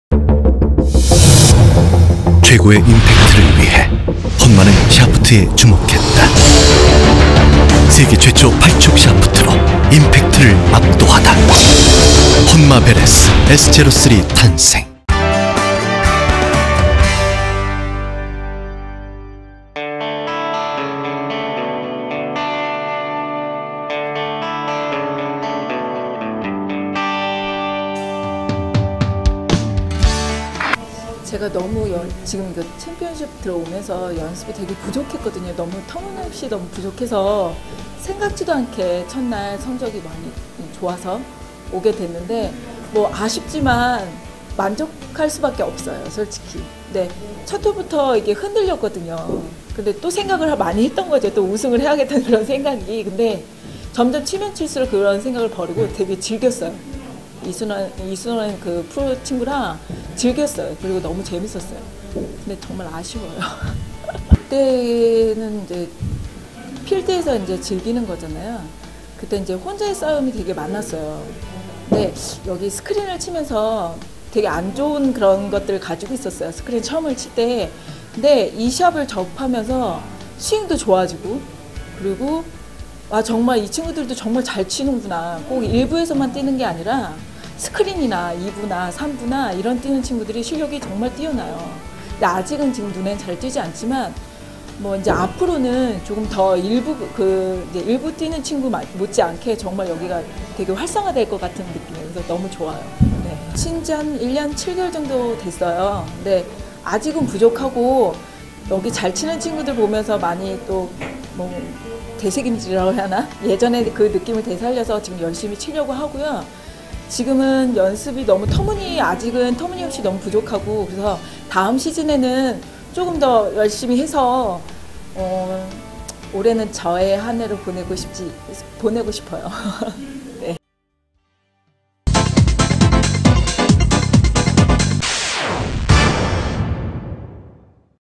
[마니아리포트]조경희 "WGTOUR, KLPGA투어만큼 활성화될 것"
[마니아리포트 조원범]한국여자프로골프(KLPGA) 투어에서 활약하던 조경희(35)가 WGTOUR로 무대를 옮겼다.

조경희는 2013-14 KT금호렌터카 WGTOUR 챔피언십에서 선두에 3타 뒤진 단독 2위에 올랐다. 이순호(28.골프존아카데미)와 공동 선두로 출발한 조경희는 최종라운드에서 1타를 잃어 아쉽게 우승을 놓쳤다. 조경희는 이날 경기에 대해 "연습이 터무니없이 부족했는데 첫 날 성적이 좋았다. 그래서 처음에는 우승해야겠다는 생각이 들었는데 치면 칠수록 즐기게 됐다"며 "정말 아쉽긴 하지만 만족한다"고 소감을 밝혔다.

2000년 KLPGA 투어에 데뷔한 조경희는 2005년 ADT캡스 챔피언십 3위, 2006년 레이크사이드 여자오픈 준우승을 기록하며 9년한 투어 생활을 했다. 조경희는 "필드 대회는 혼자 하는 싸움이었다"고 말하며 "WGTOUR에 출전하면서 스윙이 많이 좋아졌고 선수들의 실력이 매우 뛰어나서 WGTOUR가 정규투어 못지않게 활성화될 것 같다는 생각이 든다"고 말했다.

WGTOUR에 출전한지 1년 7개월이 된 조경희는 2012-13시즌부터 발군의 실력을 자랑했다. 6개 대회에 출전해 톱10에 4번 이름을 올렸고 이번 시즌에는 모든 대회(9개)에 출전해 준우승 2회를 포함, 톱10에 4차례 들었다. 조경희는 "아직은 부족하고 연습도 많이 못 하고 있지만 다음 시즌까지 열심히 준비해서 나의 해로 만들겠다"는 포부를 밝혔다.

조경희의 인터뷰를 들어보자.